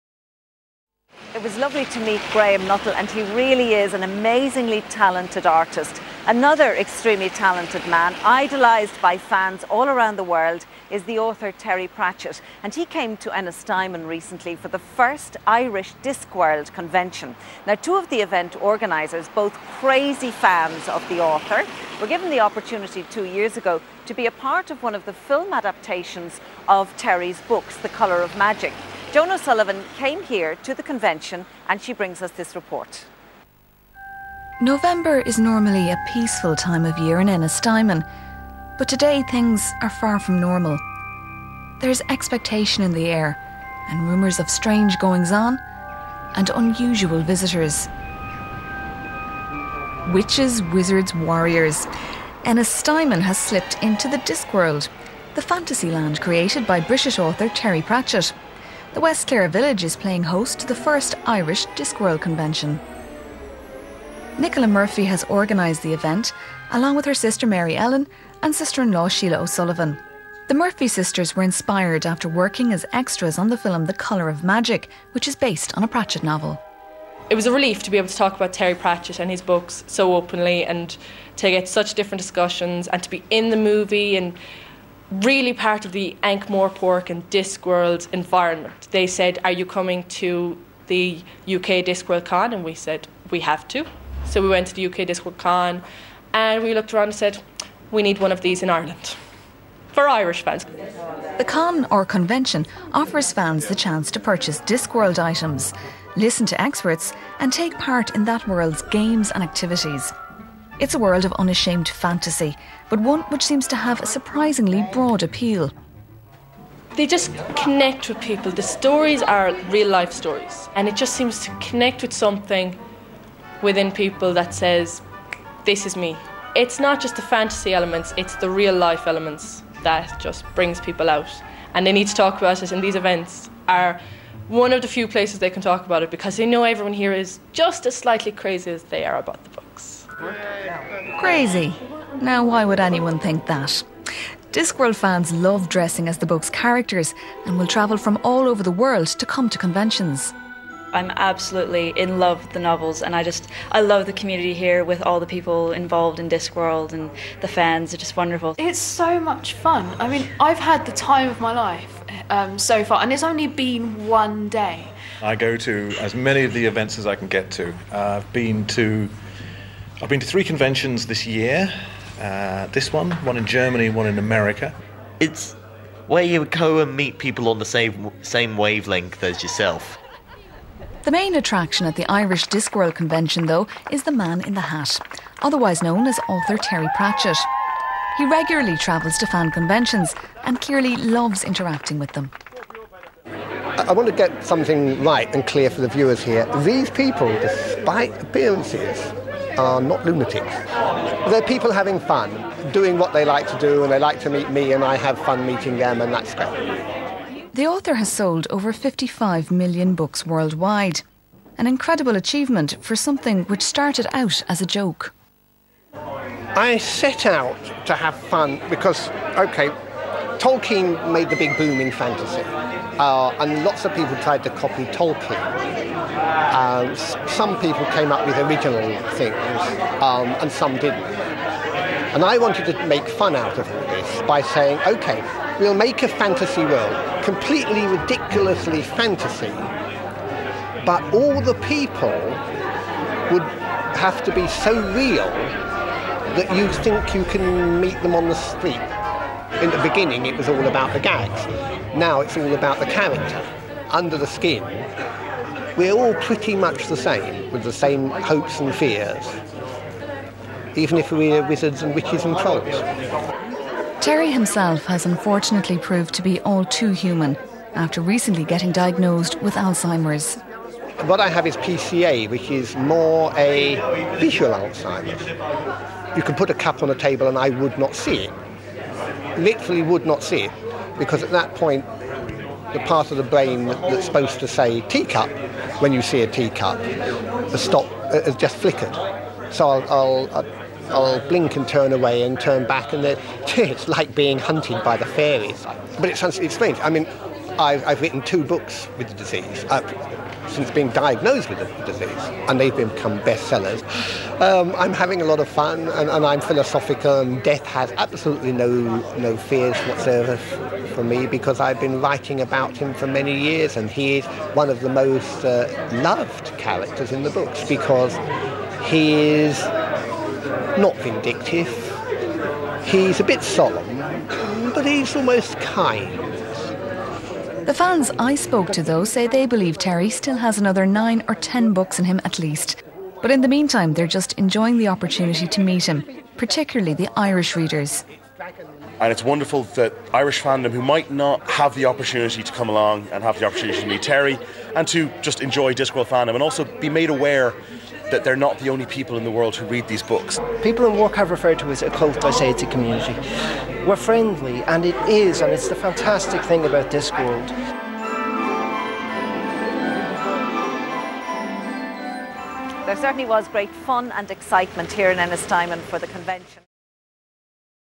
Discworld Ennistymon
Ireland's first Discworld convention with Terry Pratchett.